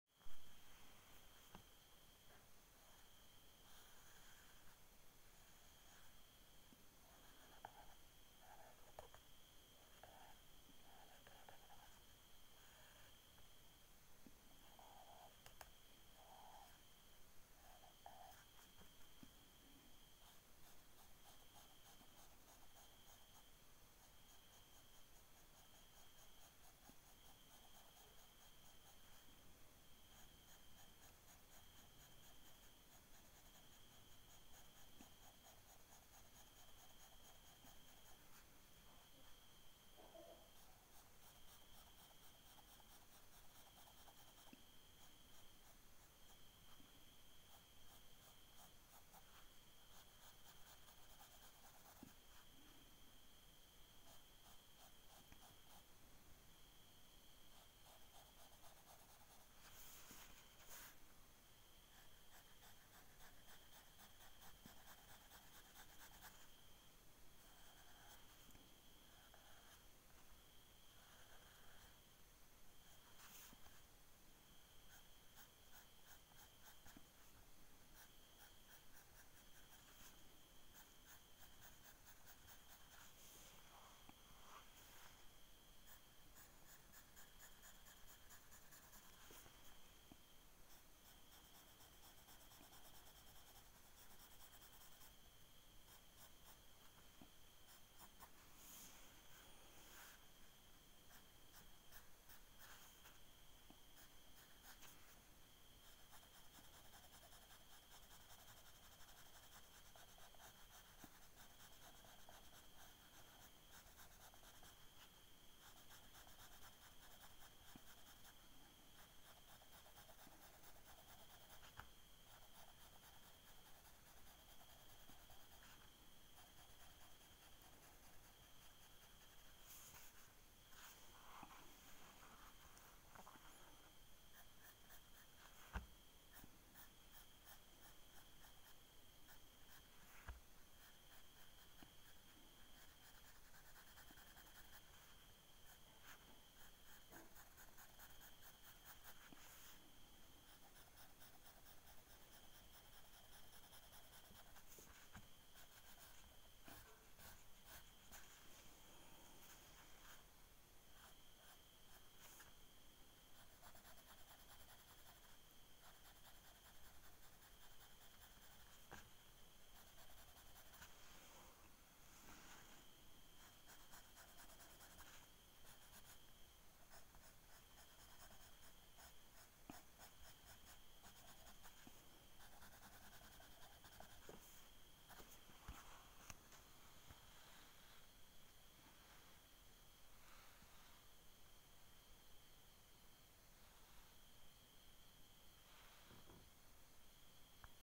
การวาดเส้น เขียนต้นไม้ #2 การวาดเส้นดินสอ การร่างภาพ
เขียนต้นไม้ การวาดเส้นดินสอ การร่างและแรเงาน้ำหนักเพิ่มรายละเอียดให้ภาพมีความชัดเจนยิ่งขึ้น